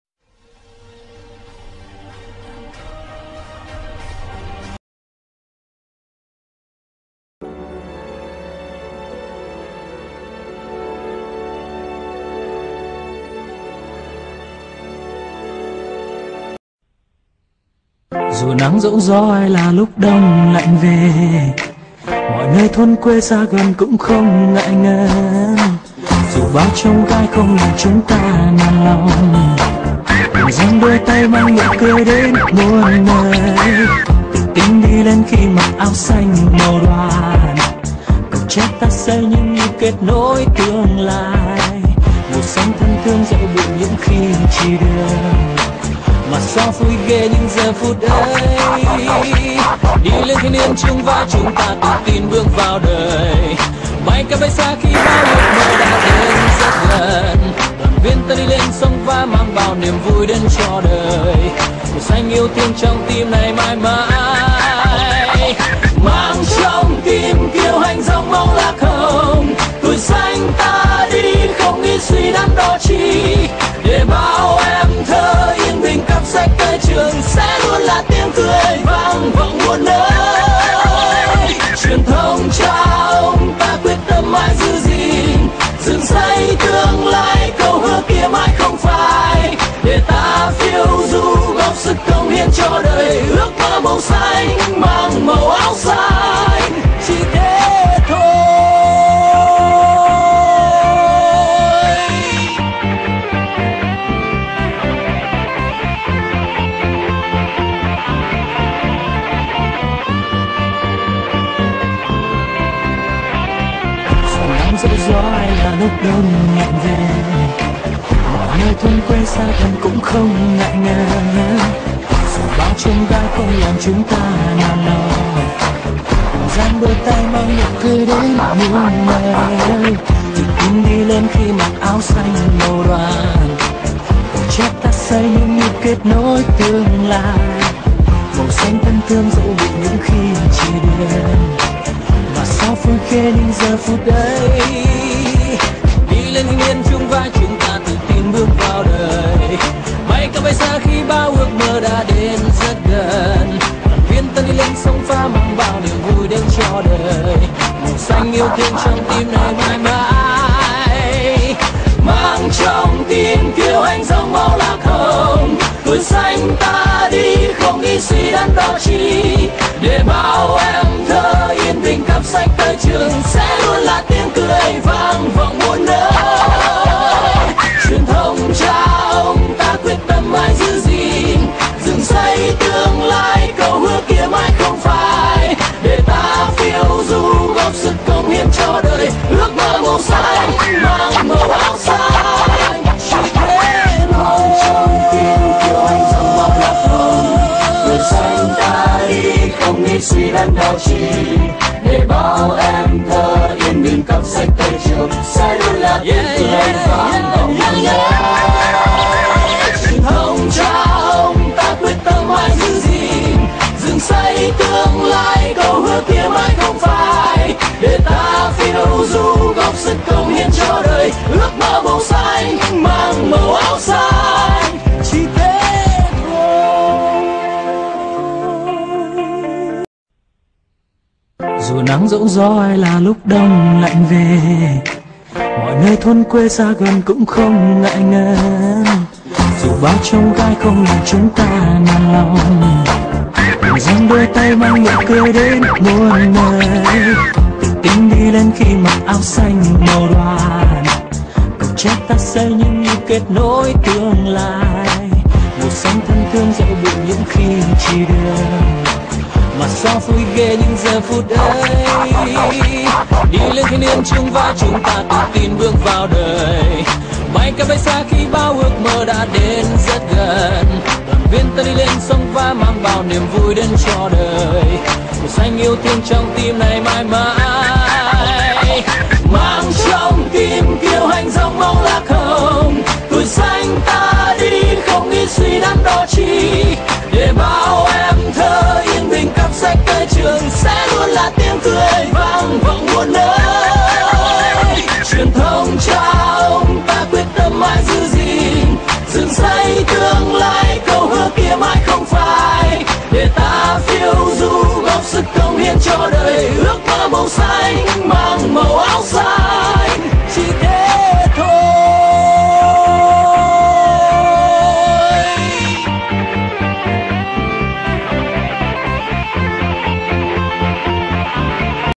Pink tv Doi Tinh nguyen xung kich tai mong son yen binh, yen bai..mpg
doi tinh nguyen xung kich hoc vien bao chi va tuyen truyen tai xa mong son huyen yen binh tinh yen bai